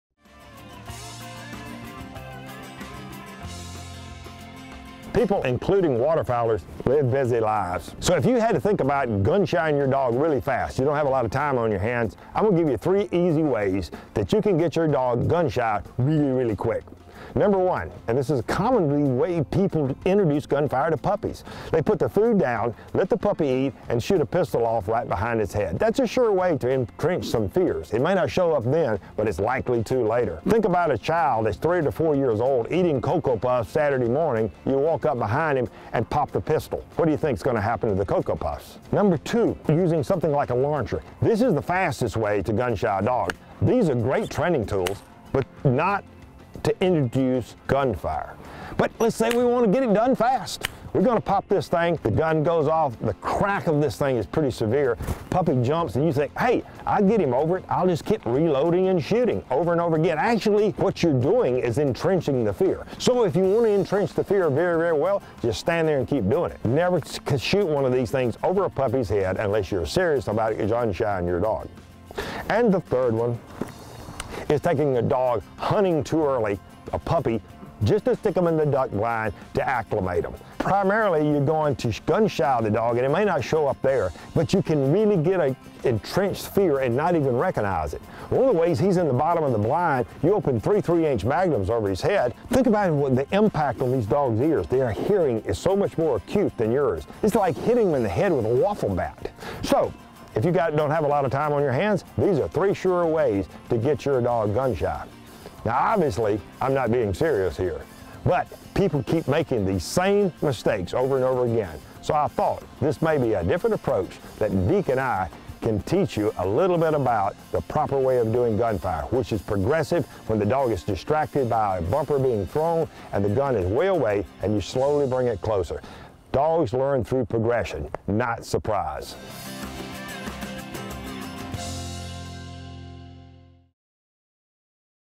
Retriever Training: Introducing Gunfire
Mike Stewart explains how to progressively introduce gunfire to your retriever.